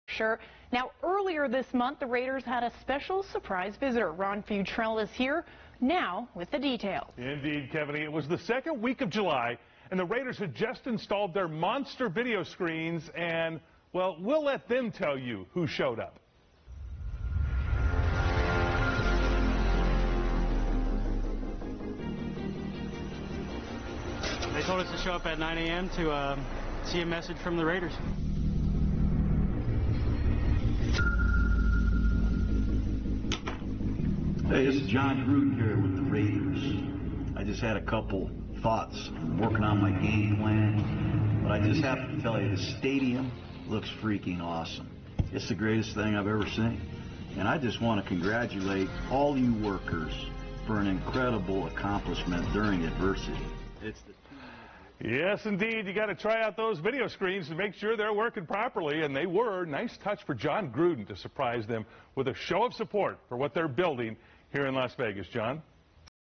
Ron Futrell reports on Jon Gruden virtual visit to Allegiant Stadium July 2020
Ron Futrell reports on Raiders coach Jon Gruden making a virtual visit to Allegiant Stadium in Las Vegas. Air Date July 25, 2020. KLAS TV Las Vegas, NV